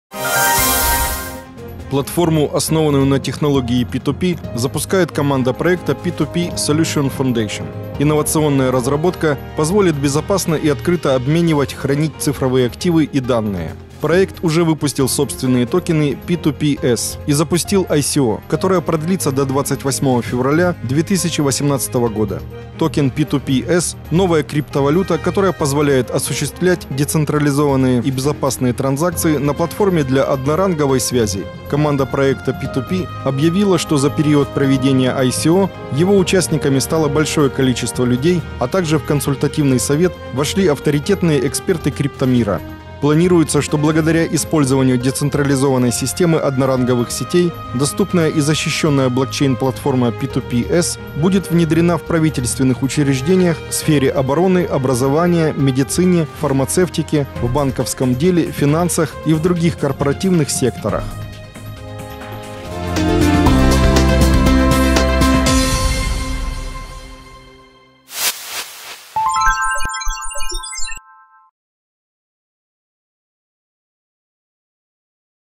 Цифровая блочная цепь с защитой и без помех, одноранговая система связи
Платформу, основанную на технологии р2р запускает команда проекта P2P SOLUTIONS FOUNDATION. Инновационная разработка  позволит безопасно и открыто обменивать, хранить цифровые активы и данные.  Проект уже  выпустил собственные токены P2PS  и запустил  ICO, которое продлится до 28 февраля 2018 года. Токен P2PS, новая криптовалюта, которая позволяет осуществлять децентрализованные и безопасные транзакции   на платформе для одноранговой связи. Команда проекта р2р  объявила, что за период проведения  ICO его участниками стало большое количество людей, а также в консультативный совет вошли авторитетные эксперты криптомира.
Планируется, что благодаря использованию децентрализованной системы одноранговых сетей, доступная и защищенная блокчейн-платформа P2PS будет внедрена в правительственных учреждениях,  сфере обороны, образования, медицине, фармацевтике, в банковском деле, финансах  и в других корпоративных секторах.

Для получения дополнительной информации и покупки токенов P2PS, перед их продажей посетите сайт

Инфо: